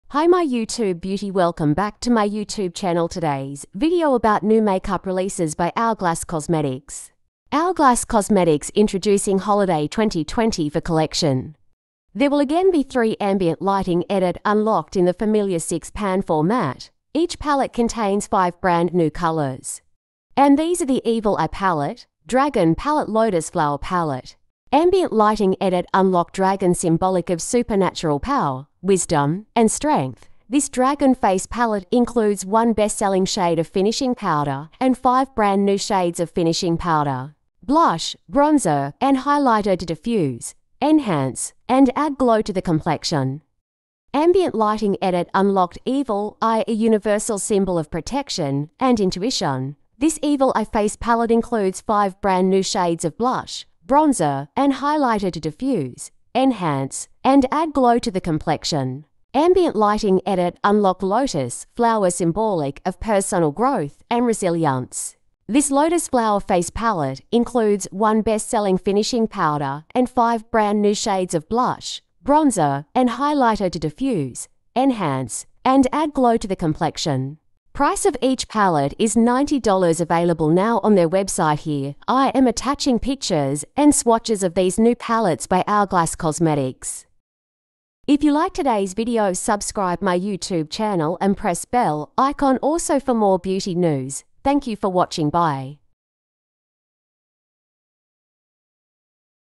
New!Hourglass Cosmetics Holiday 2024 Collection|Hourglass Cosmetics New Eyeshadow Palettes
Hourglass has shown the first pictures of this year's Holliday Collection.

There will again be 3 Ambient Lighting Edit Unlocked in the well-known 6 Pan format.

In each palette are 5 brand new colors.

And these are the pallets:

Dragon Palette:
●Ambient Lighting Finishing Powder: Dream Light (Cool Pearl With Soft Lavender Sheen)
●Ambient Lighting Blush (NEW): Peach Fusion (Soft Peach Pink)
●Ambient Lighting Finishing Powder (NEW): Divine Light (Translucent With Sheer Pink Sheen)
●Ambient Lighting Blush (NEW): Mauve Haze (Soft Plum)
●Ambient Lighting Bronzer (NEW): Soft Bronze (Matte Natural Bronze)
●Ambient Strobe Lighting Powder (NEW): Sunlit Strobe Light (Pale Pink Champagne)

Evil Eye Palette:
●Ambient Lighting Powder: Soft Light (Pale Peach)
●Ambient Strobe Lighting Powder (NEW): Rose Gold Strobe Light (Soft Rose Gold)
●Ambient Lighting Blush (NEW): Pink Fusion (Cool Pink)
●Ambient Lighting Blush (NEW): Oasis Glow (Shimmering Warm Apricot)
●Ambient Lighting Blush (NEW): Vibrant Coral (True Coral)
●Ambient Lighting Bronzer (NEW): Natural Bronze (Matte Medium Bronze)

Lotus Palette:
●Ambient Strobe Lighting Powder (NEW): Gilded Strobe Light (Warm Bronze Gold)
●Ambient Lighting Blush (NEU): Desert Flush (Deep Beige Peach)
●Ambient Lighting Finishing Powder: Eternal Light (Golden Amber)
●Ambient Strobe Lighting Powder (NEU): Radiant Rose Strobe Light (Shimmering Copper Rose)
●Ambient Lighting Blush (NEU): Red 0 (True Red)
●Ambient Lighting Bronzer (NEU): Bronze Heat (Matte Deep Bronze)

There is also a limited brush in 2 designs.

The Ambient Lighting Edit Brush is available on double sides and matching 2 of the pallets:

• Ambient Lighting Edit Brush - Dragon°
• Ambient Lighting Edit Brush - Evil Eye°

Available Now on their website
Price of each Palette is $90
Images Courtesy of Brand